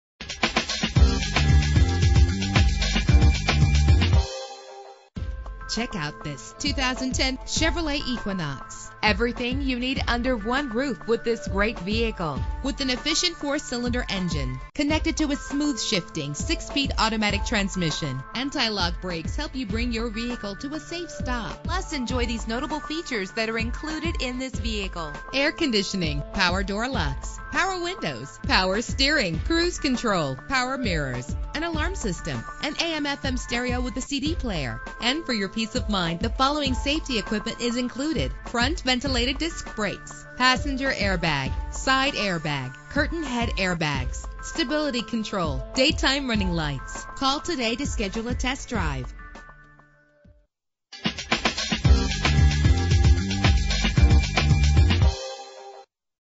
2010 Chevrolet Equinox - Sumter SC
Year: 2010
Make: Chevrolet
Model: Equinox
Trim: LT
Engine: 2.4 liter inline 4 cylinder 16 valve
Transmission: 6-Speed Automatic
Color: Silver
Mileage: 33461
Address:
1260 Broad St